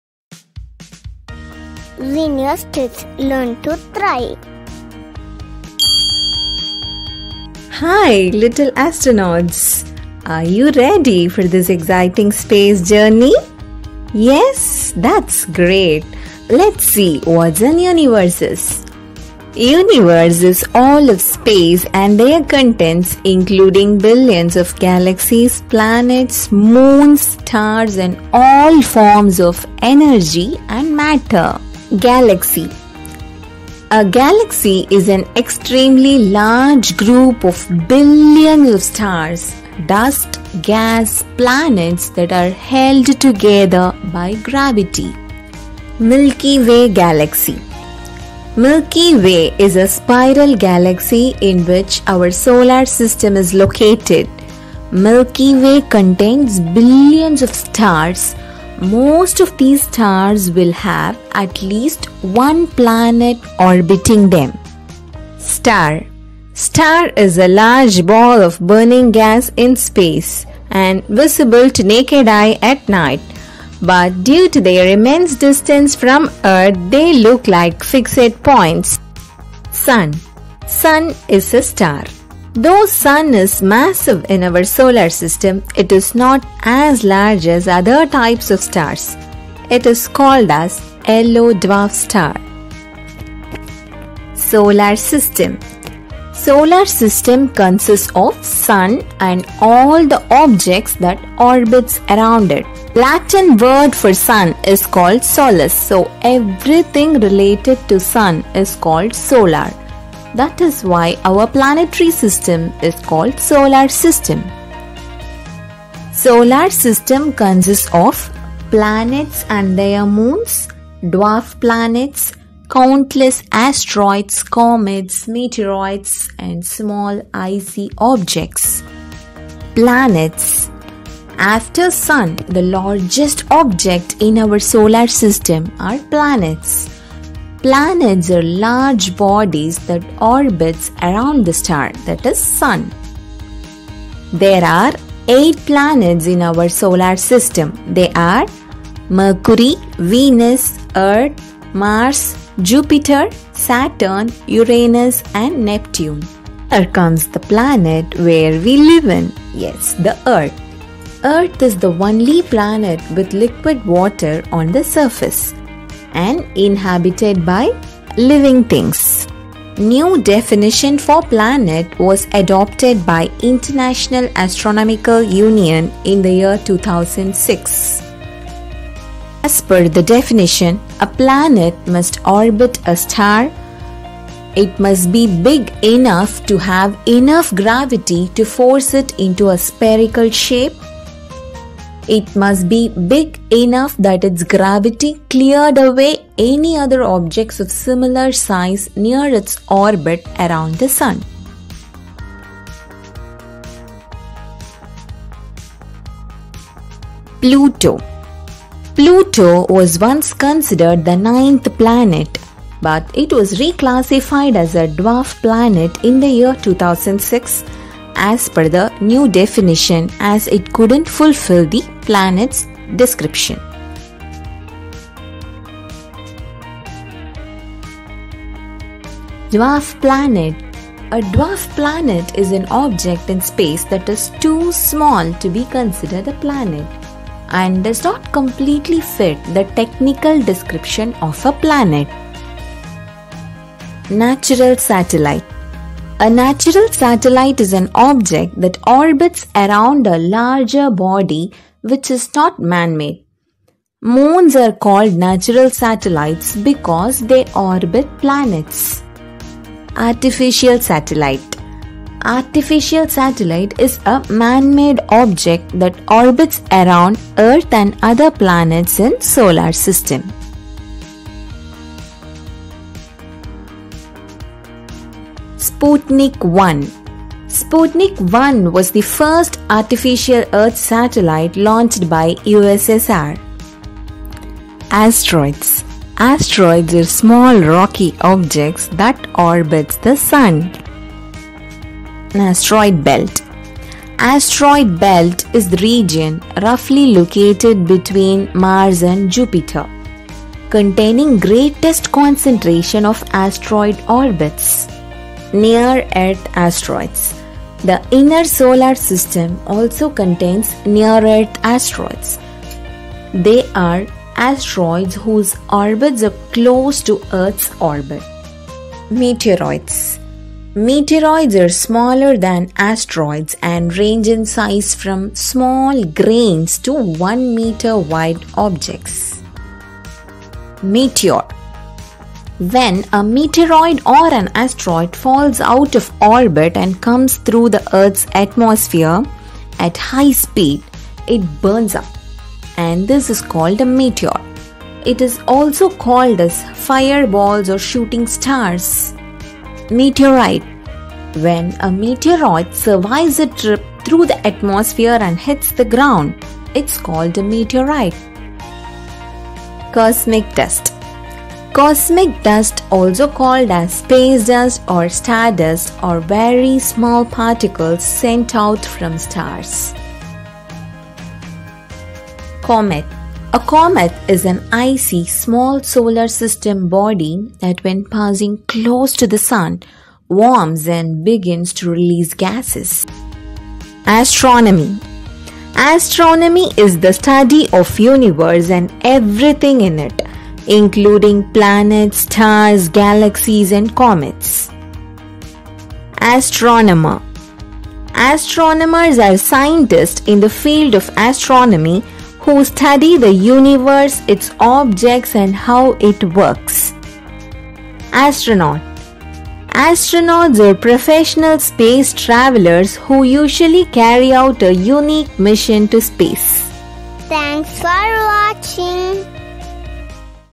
Let's Explore our universe | Virtual Journey to Space | Science video for kids
Let's Explore our universe | Virtual Journey to Space

The universe is all of space and its contents including billions of galaxies, planets, moons, stars and all forms of energy and matter. This is a Science Educational video for kids. Through this video, Kids will learn about

Universe
Galaxy
Milky way galaxy
Star
Sun
Planets
The new definition of Planet
Earth
Pluto - Dwarf planet
Natural and Artificial Satellite
Asteroids
Meteoroids
Meteorite
Cosmic dust
Comet
Astronomy
Astronomer
Astronaut